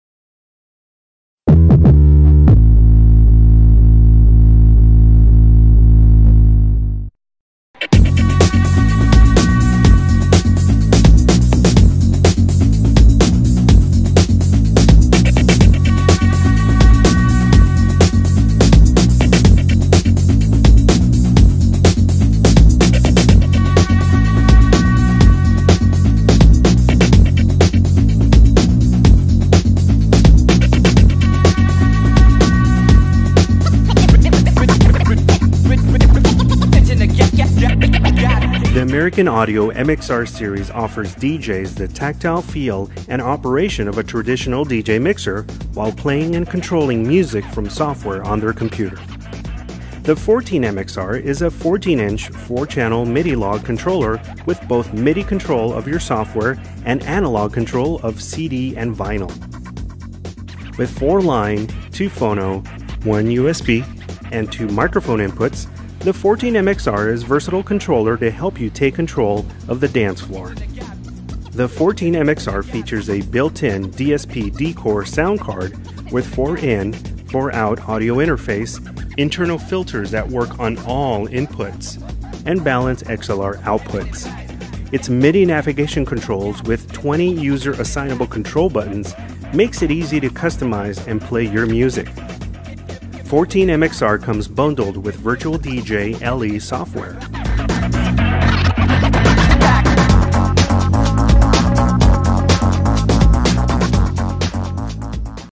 American Audio 14 MXR
4 Phono, 2 Line, 2 Aux & 2 Microphone inputs
4 MIDILOG™ Channels (compatible with any "MIDI Software" or Analog Inputs - such as CD Players, Turntables, etc.)
Integrated 4x4 soundcard
Bundled with Virtual DJ LE
Integrated browser for easy access to your music library
Sync button
Multifunctional Shift button
Innofader compatible Crossfader
Crossfader curve adjustment
Crossfader assignable to each channel
3 Cue buttons on each channel
Rotary 3 band EQ for each channel
High and Low Pass filter for each channel
Microphone gain control
Talkover button
Microphone 3-band EQ
Booth output
Balanced XLR outputs
Multi-voltage: CA 100 V~240 V 50/60 Hz